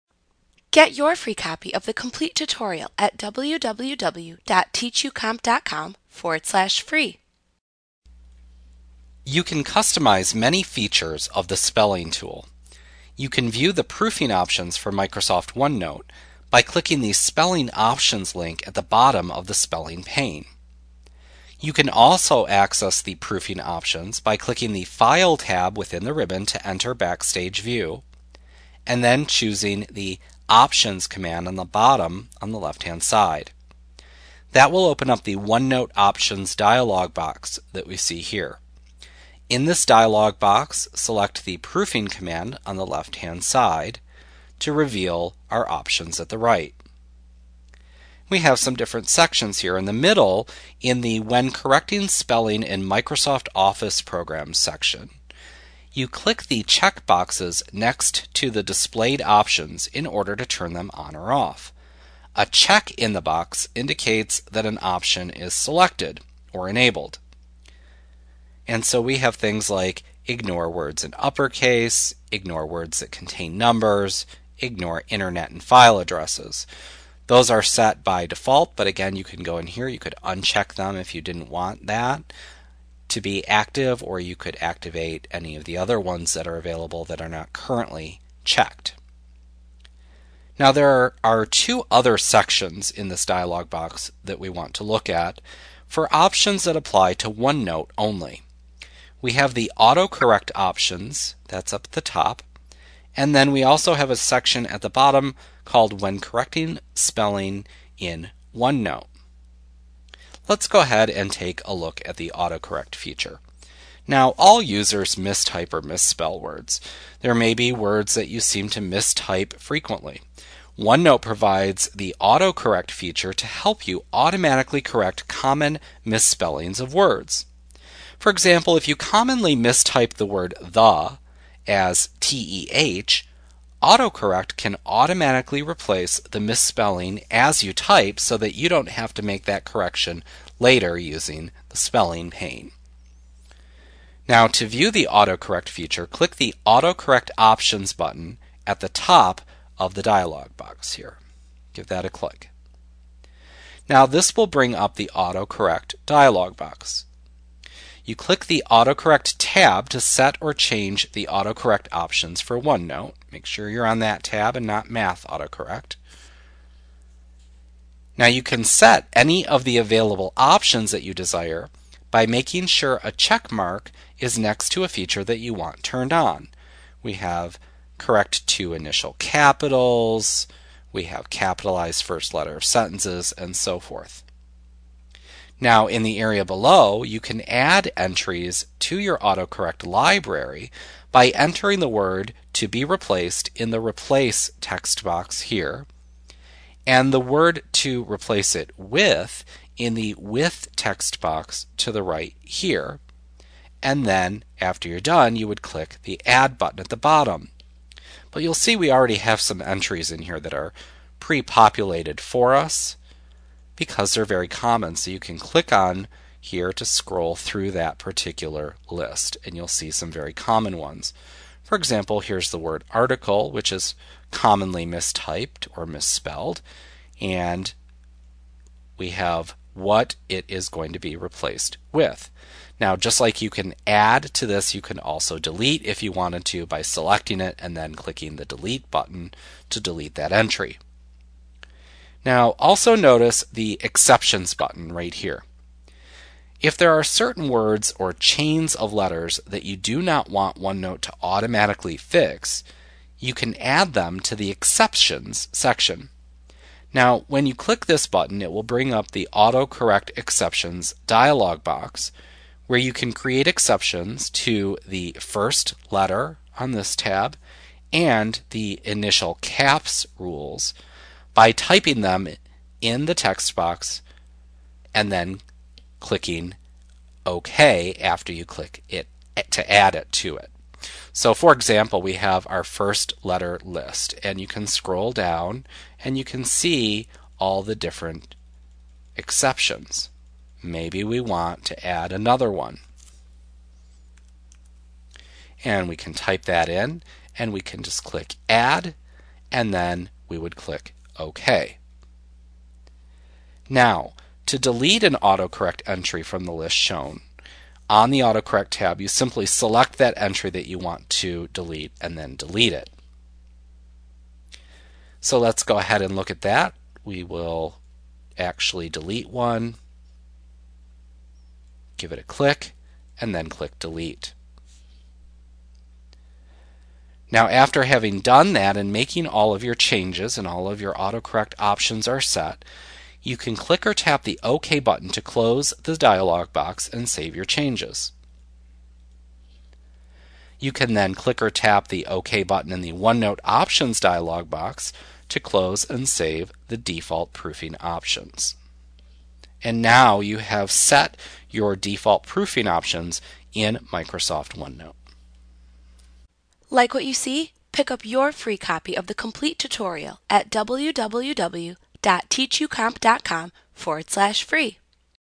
OneNote 2013 Tutorial Setting Default Proofing Options Microsoft Training Lesson 4.4
Learn about Setting Default Proofing Options in Microsoft OneNote A clip from Mastering OneNote Made Easy. the most comprehensive OneNote tutorial available. Visit us today!